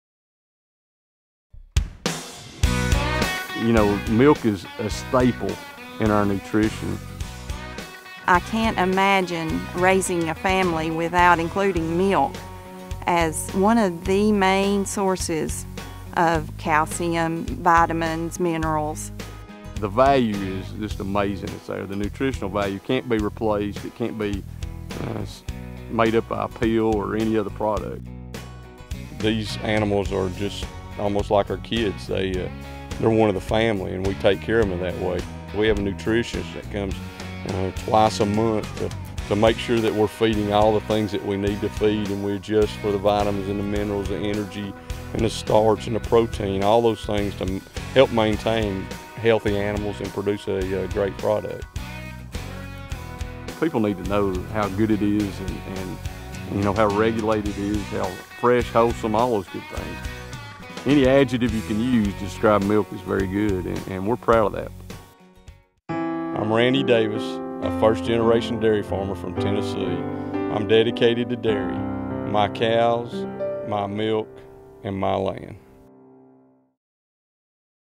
Meet the Davis Dairy Farm Family from Tennessee
Randy Davis is a first-generation dairy farmer, but is already building a legacy. Davis did not become a full-time dairy farmer until about 20 years ago, after spending a decade as a high school teacher and coach. “The cows and heifers are really a part of our family and we treat them that way.” Davis said when he first started the dairy farm he did not anticipate it would turn into a full-time vocation. Today, Davis Bros. Dairy in Philadelphia, Tenn., is home to more than 1,100 cows on about 1,175 acres.